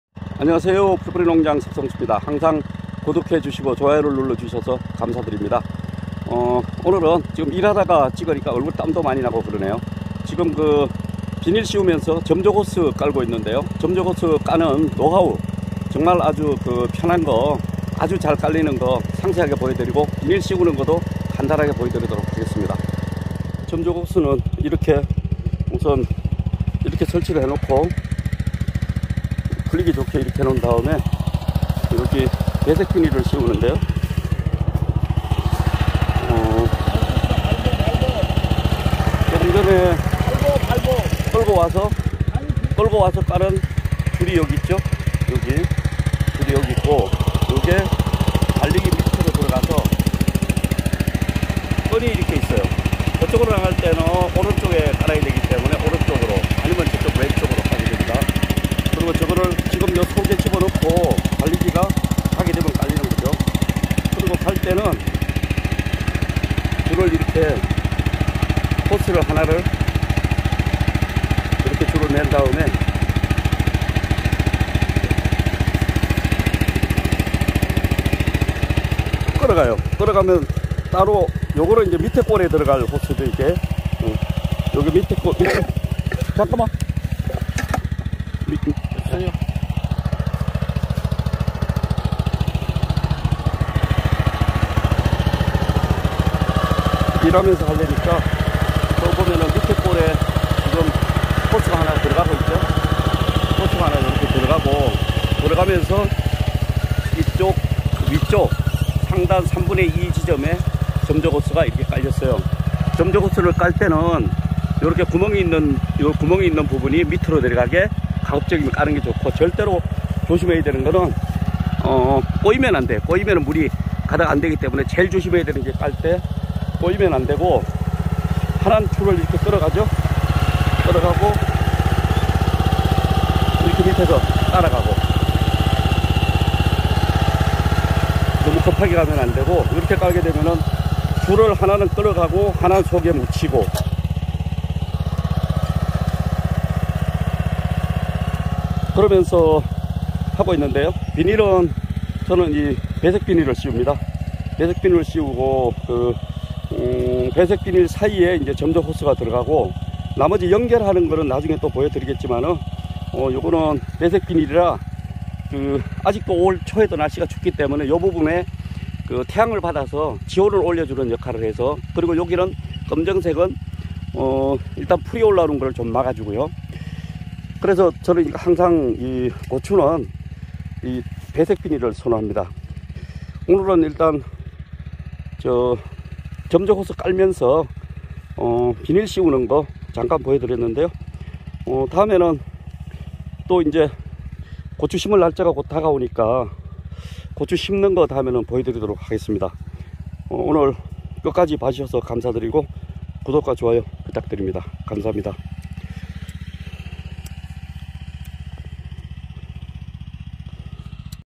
고추밭 비닐씌우고 점적호스깔기 비닐 종류 [고추재배방법]
고추밭 비닐씌우면서 점적호스 쉽게 설치하는 방법 공유해 봅니다.